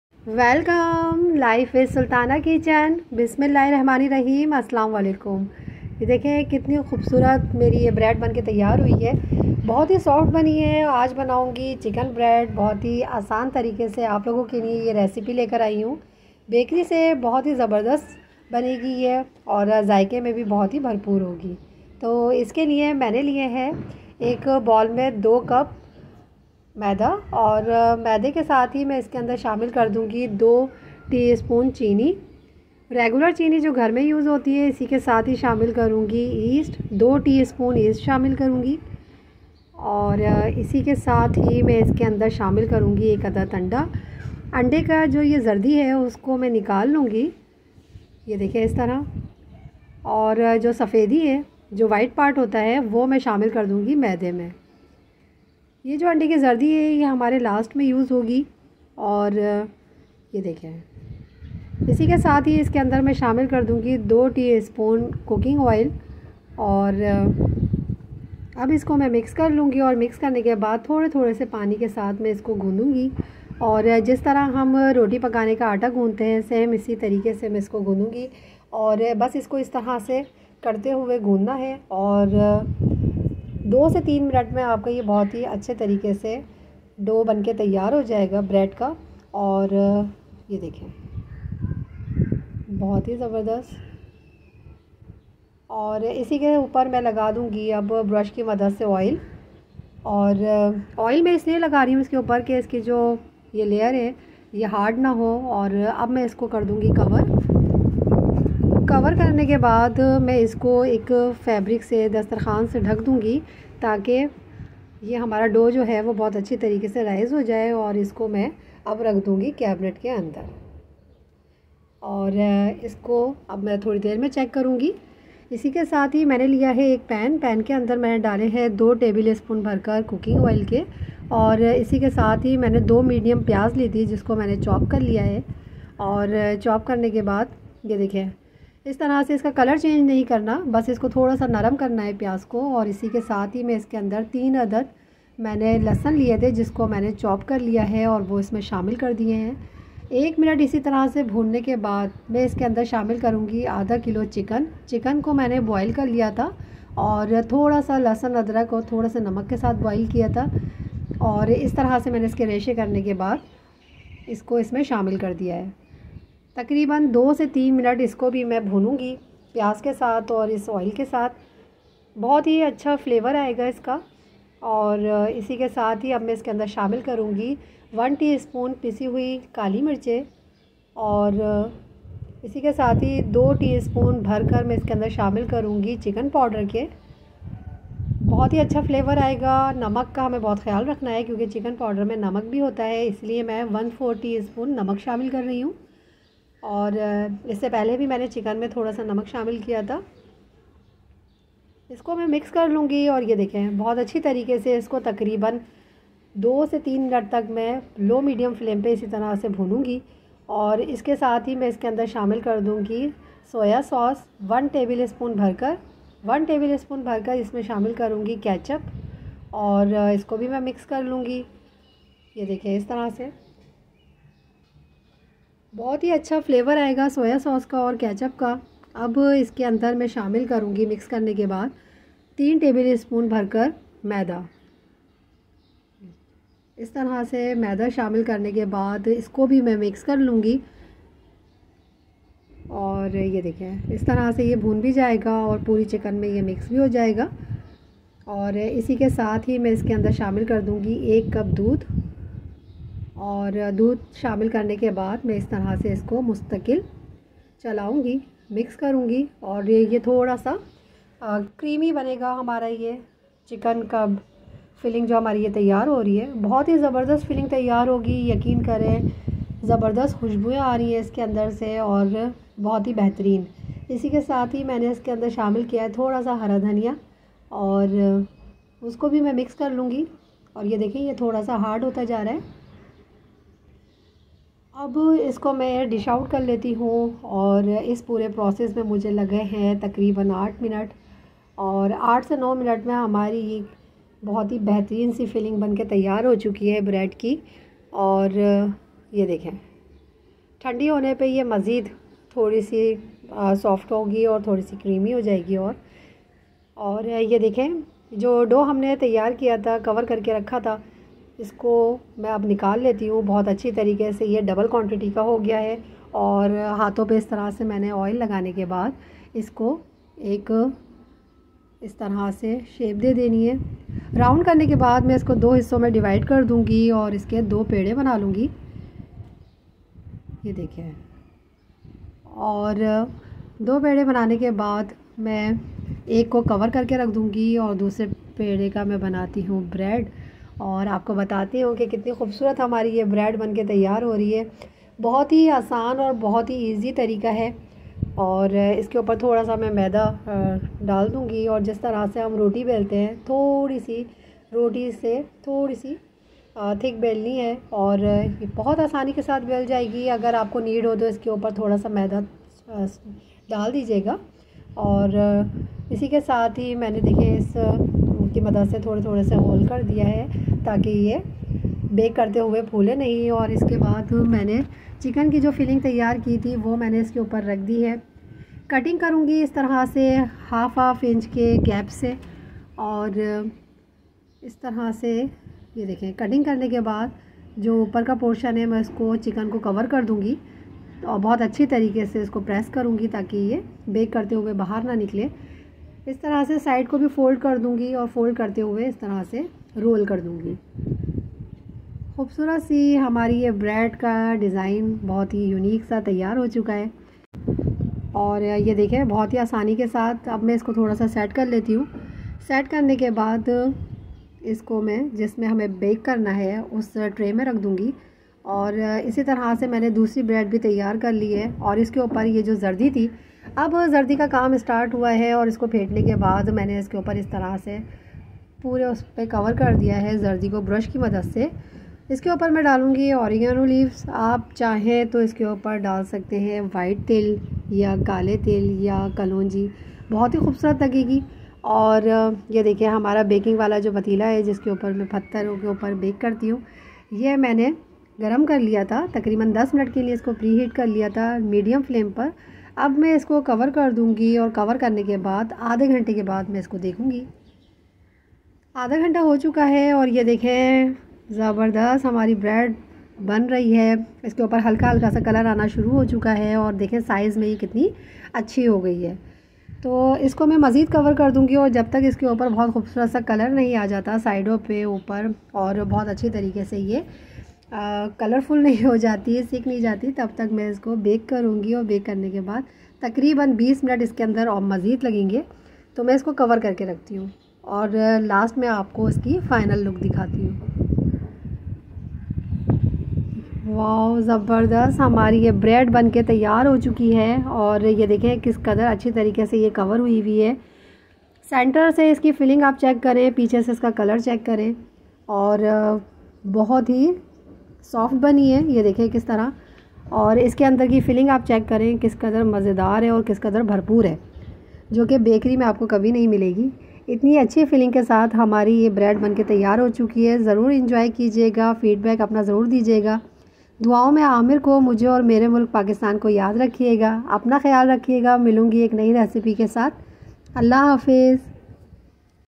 Chicken Bread Recipe Without Oven / Bakery Style No Oven Chicken Bread Recipe
Chicken Bread
Recipe

Ingredients:
Dough:
-Maida (Flour) 2 cups
-Khameer (Yeast) instant 2 tsp
-Cheeni (Sugar) 2tsp
-Pani (Water) warm 5-6 tbs or as required

Chicken filling:
-Oil 3 tbs
-Pyaz (Onion) Chopped 2 medium
-Garlic chopped 1 tbsp
-Chicken powder 2tbsp
-Soya sauce 1tbsp
-Ketchup 1tbsp
-Maida (All-purpose flour) ¼ cup
-Doodh (Milk) 1 cup
-Chicken Boiled & shredded ½ kg
-Dhania (Green coriander) 2 tbs
Assembling:
-Chicken filling
- Anda (Egg) yolk 1+ water 1 tsp
- Oregano as required

Directions:
Dough:
-In bowl, add all-purpose flour,instant yeast,sugar and salt,egg white mix well.
-add oil and mix it with the help of hands.
-Add warm water (as required), mix and knead well until all ingredients are combined.
-Take dough,punch and knead it again and grease the dough with oil.
-Cover and leave the dough for 1 & 1 ½ hour in a warm place.
-Knead dough again,cover and rest again for 30 minutes.
-Bread dough is ready.
Chicken filling:
-Boil the chicken with salt and ginger garlic paste shredded and set aside.
-In wok,add oil,let it melt then add onion and sauté little.
Add garlic,soya sauce, ketchup, chicken power and black paper
-Add all-purpose flour and sauté until its color changes.
-Remove from heat,add milk,whisk continuously to avoid any lumps formation.
-Bring back on heat,add salt and mix it.
-Add boiled and shredded chicken,